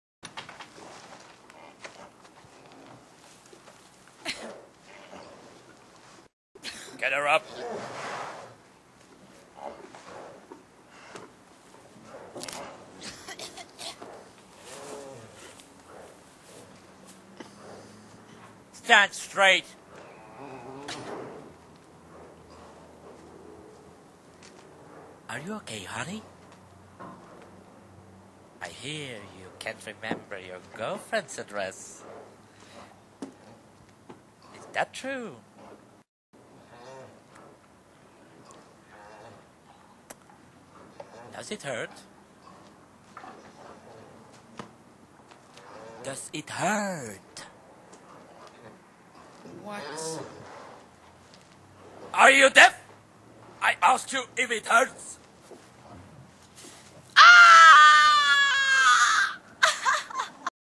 Sigarenfoltering op Saartje Vandendriessche
Nihoul ondervraagt jonge vrouw.